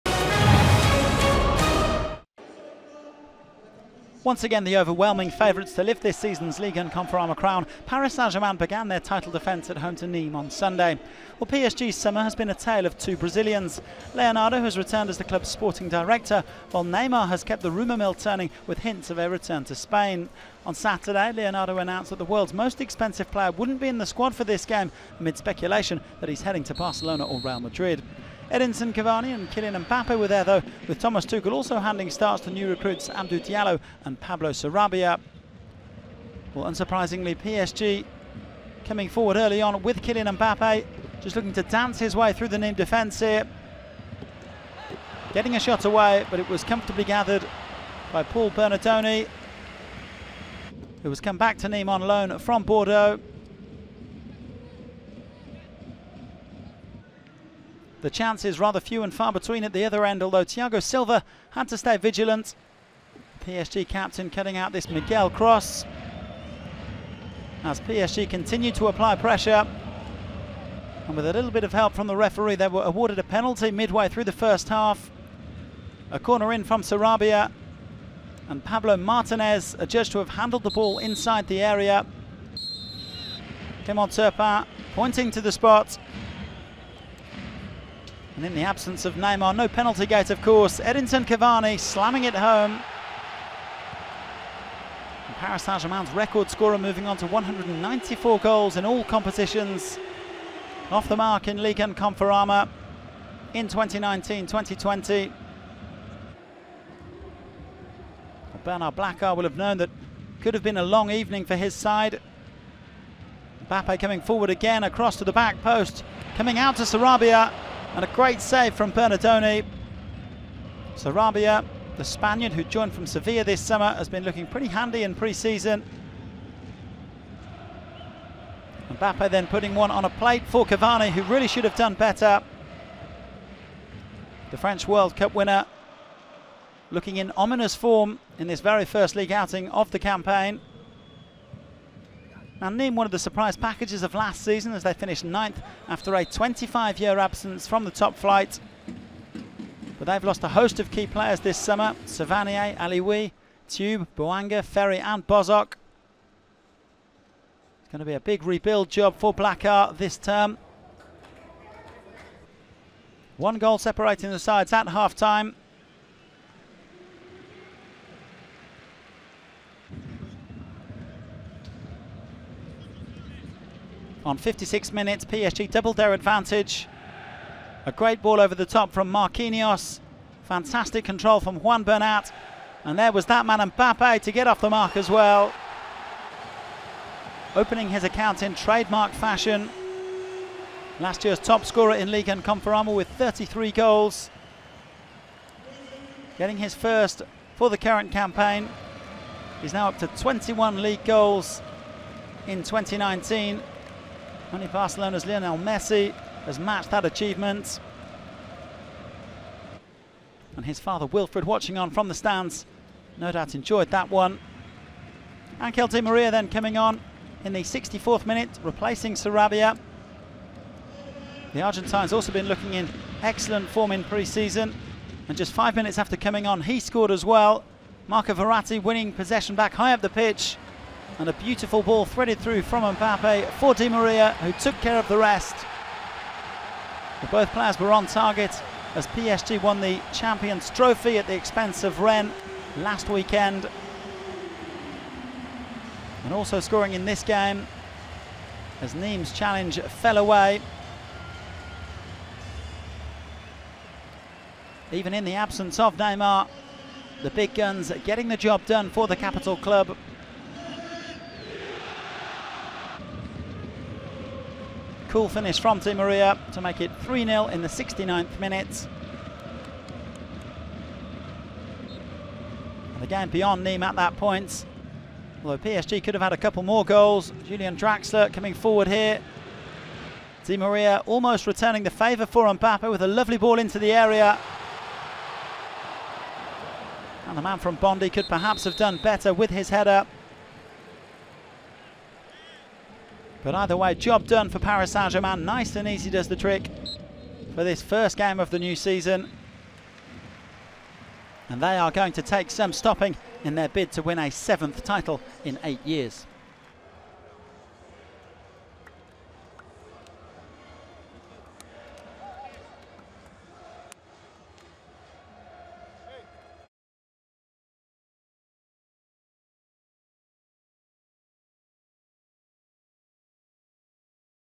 Paris Saint-Germain - Nîmes Olympique ( 3-0 ) - Highlights - (PARIS - NIMES) / 2019-20
Paris Saint-Germain vs Nîmes Olympique Highlights ( 3-0) in video.

Ligue 1 Conforama - Season 2019/2020 - Week 1 Parc des Princes - Sun, 11 Aug 2019

Goals : Edinson CAVANI (24' pen - Paris Saint-Germain) / Kylian MBAPPE (56' - Paris Saint-Germain) / Angel DI MARIA (69' - Paris Saint-Germain)

Nîmes Olympique Team line-up : 4 - Pablo MARTINEZ / 7 - Romain PHILIPPOTEAUX / 10 - Zinedine FERHAT / 14 - Antonin BOBICHON / 15 - Gaëtan PAQUIEZ / 18 - Théo VALLS / 20 - Renaud RIPART / 23 - Anthony BRIANCON (c) / 26 - Florian MIGUEL / 29 - Sofiane ALAKOUCH / 30 - Paul BERNARDONI